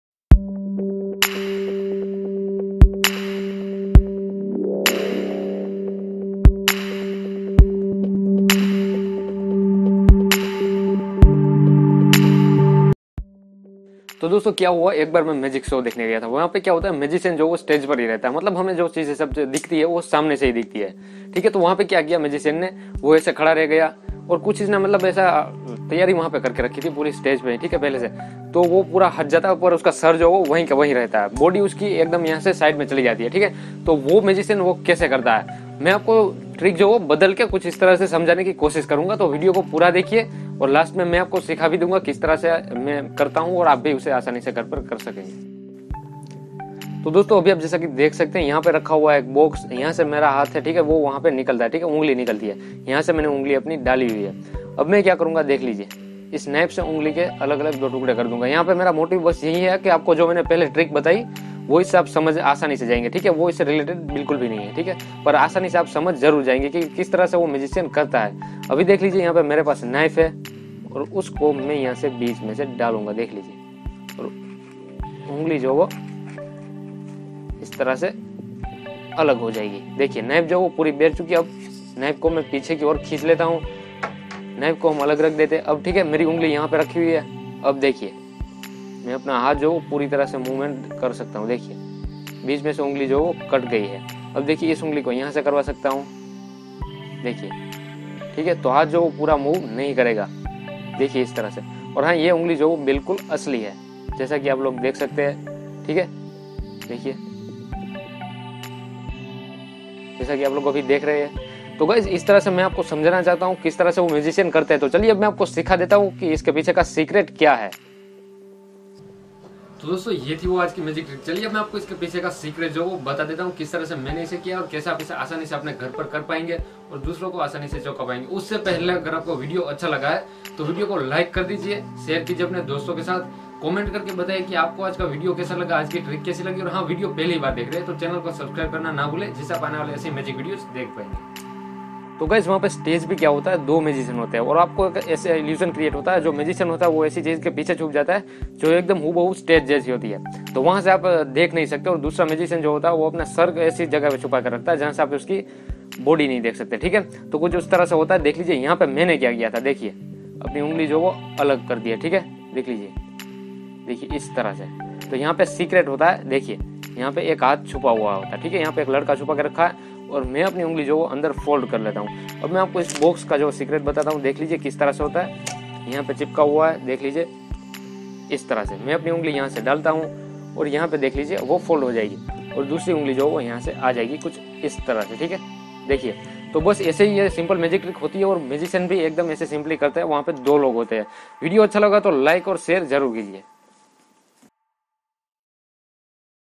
ऊँगली से अनोखा जादू Magic trick With Finger in Hindi
Hello Guy Aaj me aapko ek esi magic trick sikhaunga jo maine magic show me dekhi thi ha uss jesi to nahi par thoda badlav karke aap logo ko sikhaunga to ye magic trick ah=gar achhi lage to video ko like,share kare and subscribe for new update.